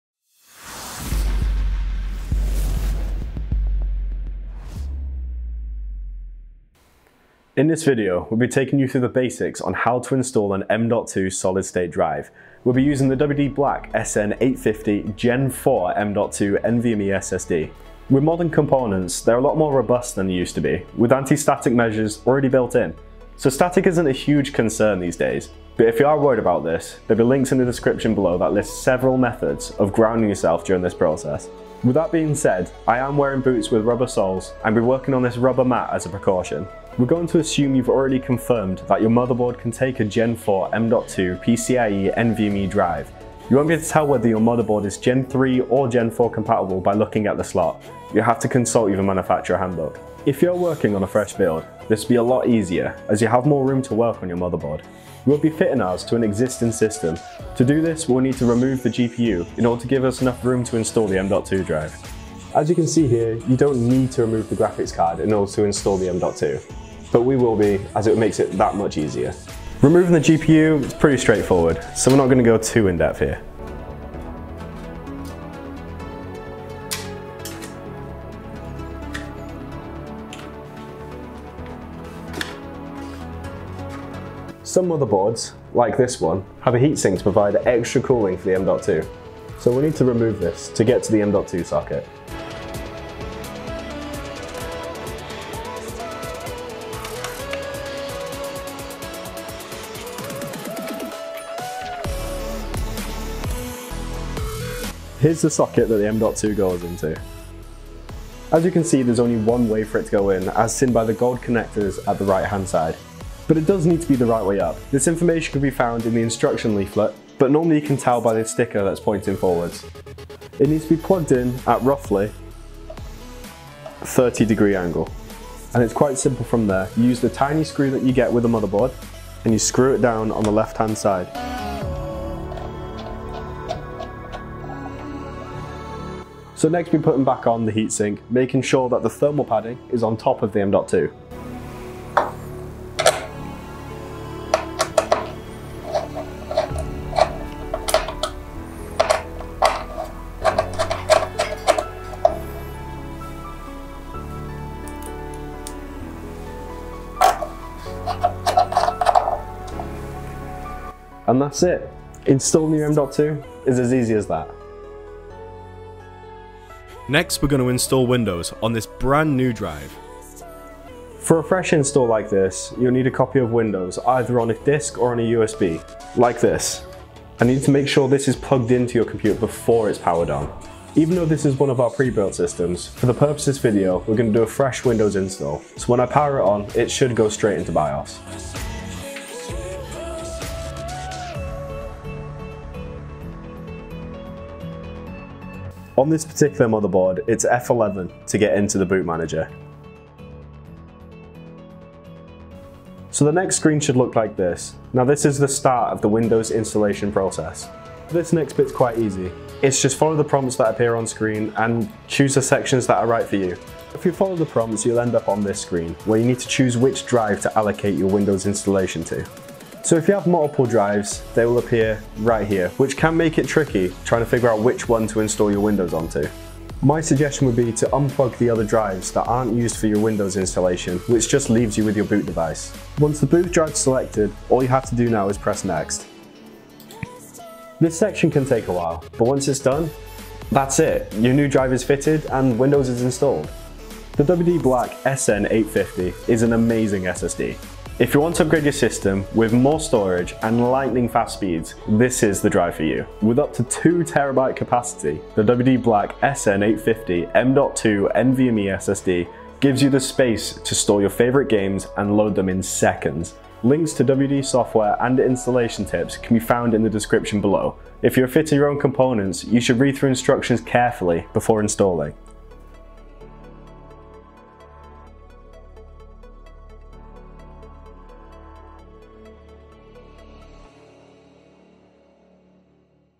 How to Install and Format Your M.2 NVMe SSD | Installing Windows
0:00 Intro
0:36 Getting Ready To Install
1:02 Removing The GPU
1:24 Installing the M.2
2:50 Installing Windows onto the drive
4:04 Outro